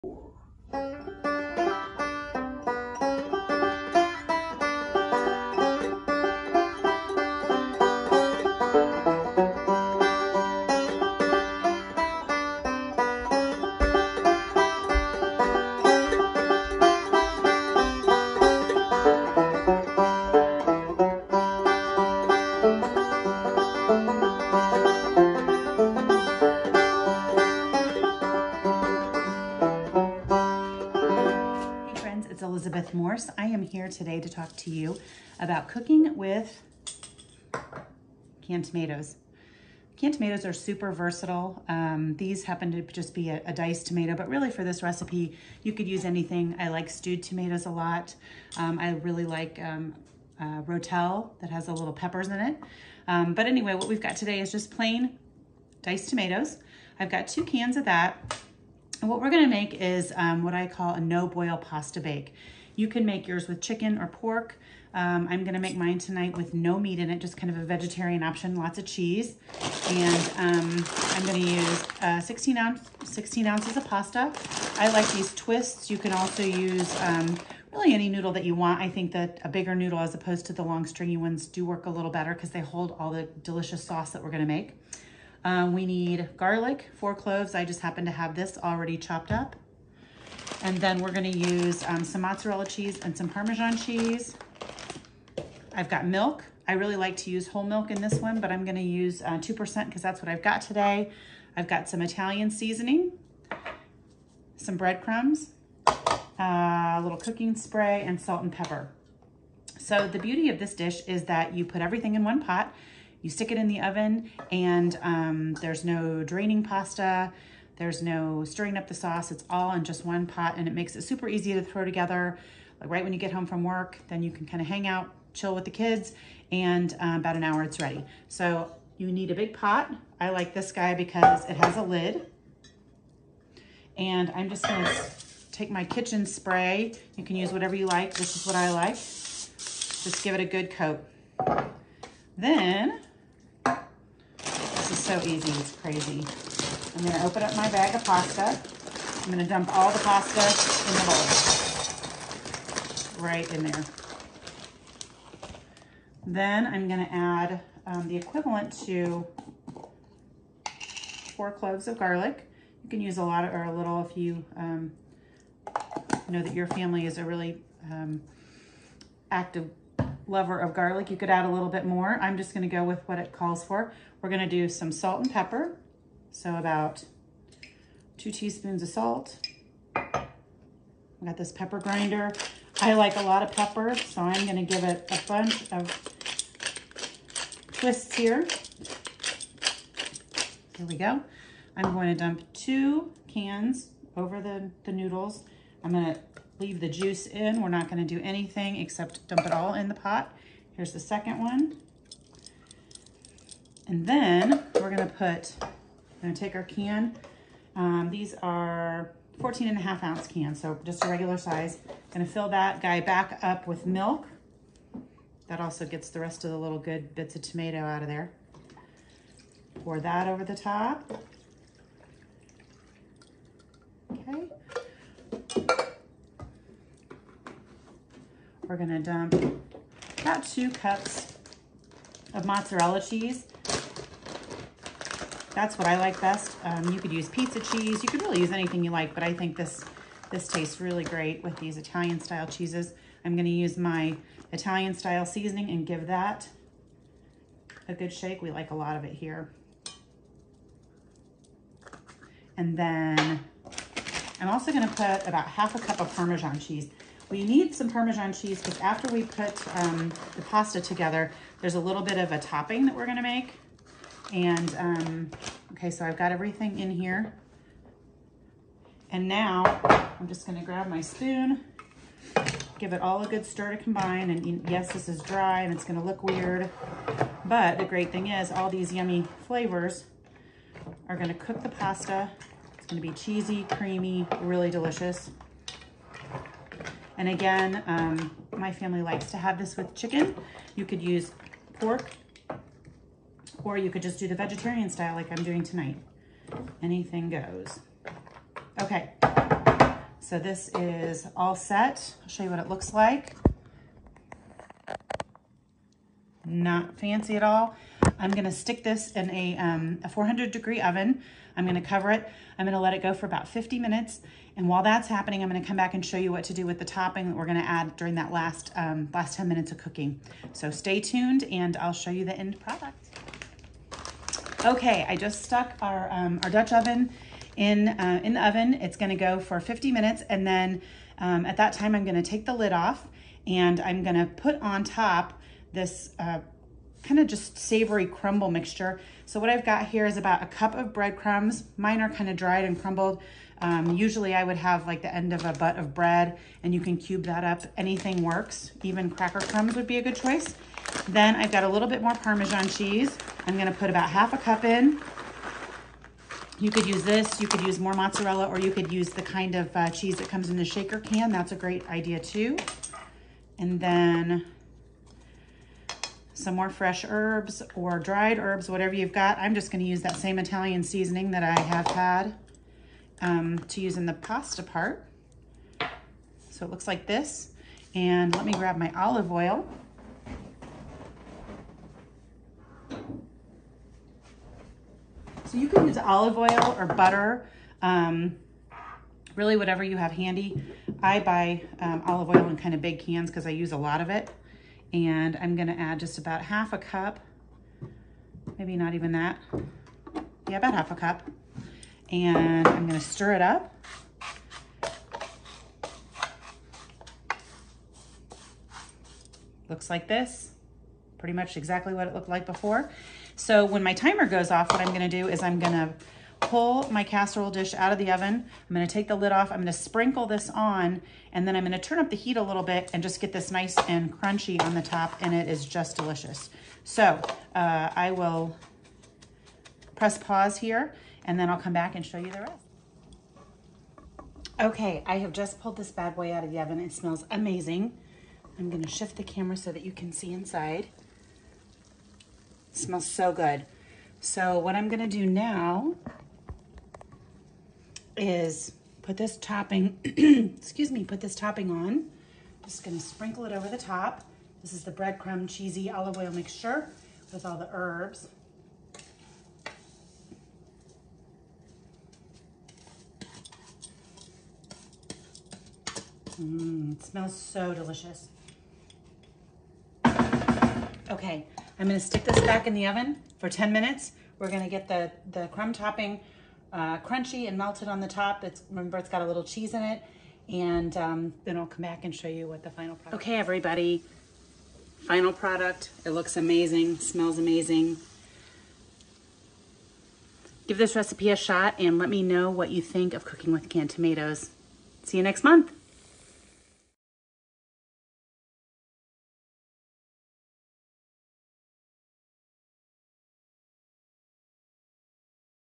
Cooking with Canned Tomatoes!
Enjoy a delicious and simple one pot meal featuring canned tomatoes! Inexpensive and ready in no time, this is a cheesy dinner the whole family will love.